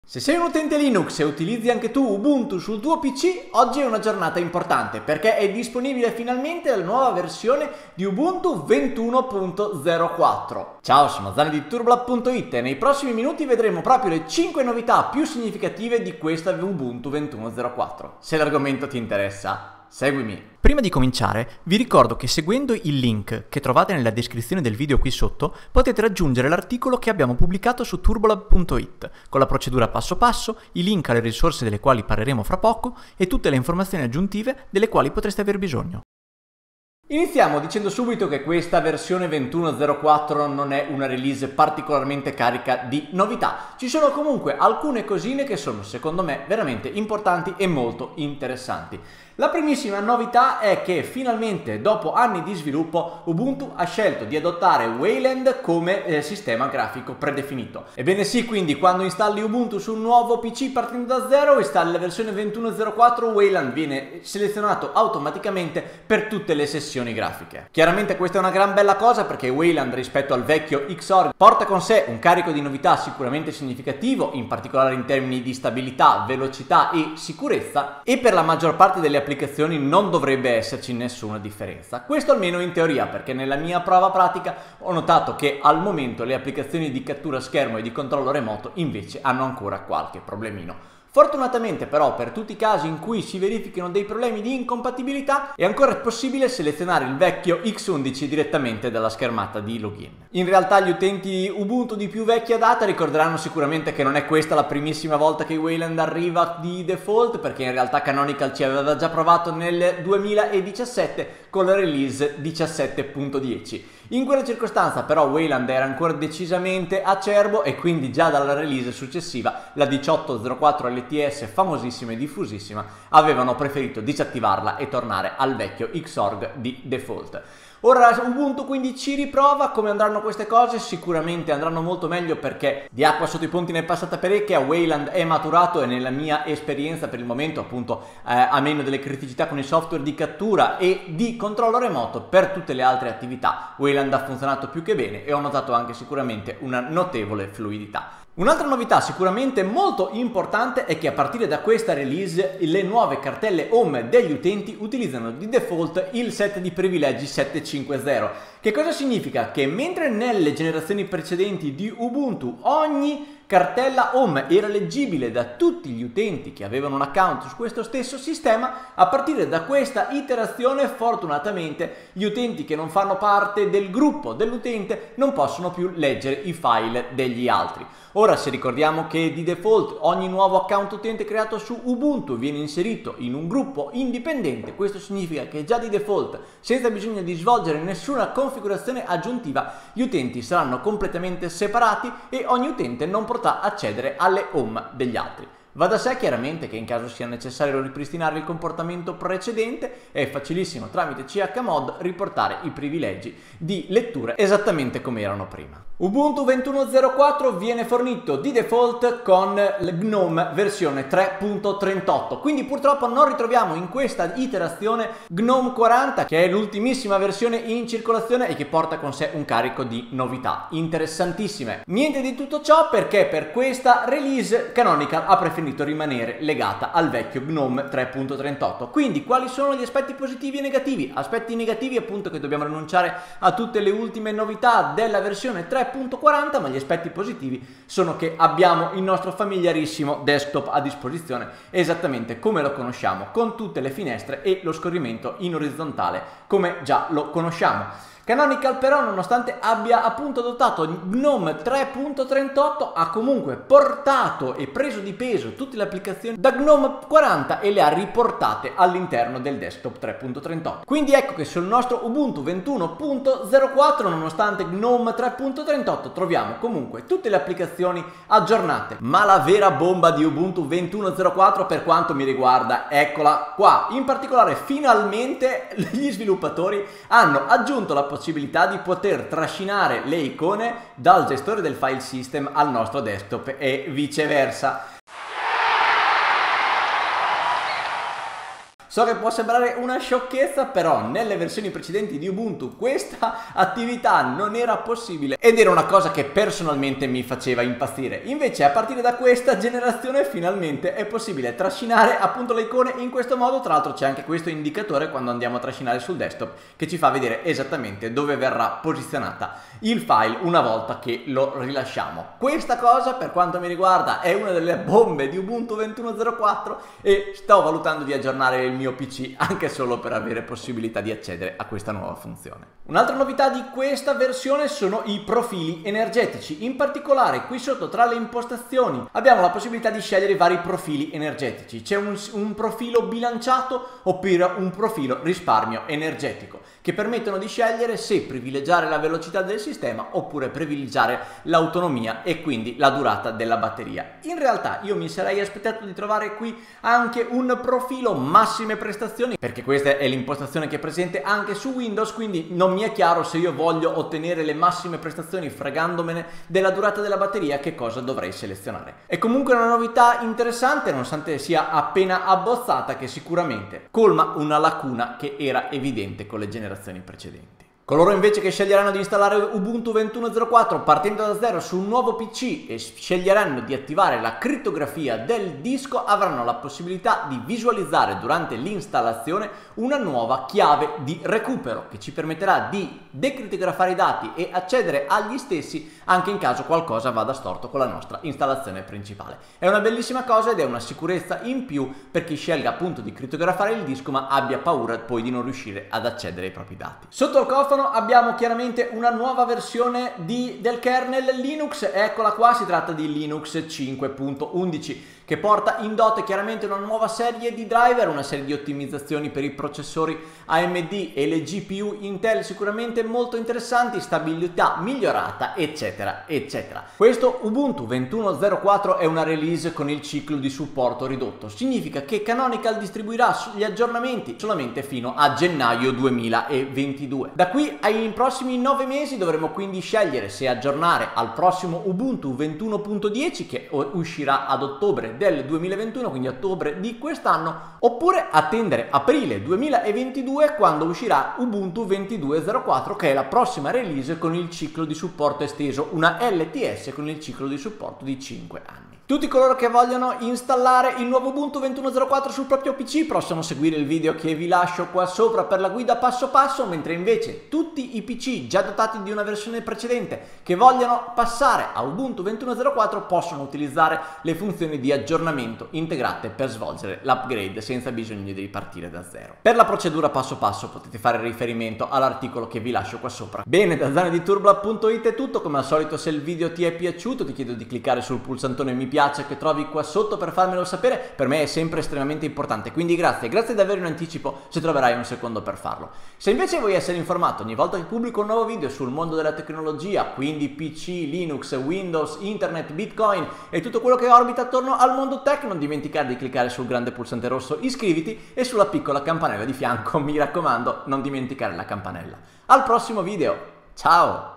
🐧 TOP 5 NOVITÀ in UBUNTU 21.04 (Hirsute Hippo, versione 2021)
È finalmente disponibile per il libero download il nuovo Ubuntu 21.04 (nome in codice: Hirsute Hippo). L'edizione di aprile 2021 porta in dote alcune novità decisamente interessanti. Manca GNOME 40, ma in compenso ci sono Wayland di default (di nuovo?) il kernel Linux 5.11, e -incredibile!- la possibilità di trascinare le icone sul desktop. Vediamo quindi in dettaglio le 5+ principali novità di Ubuntu 21.04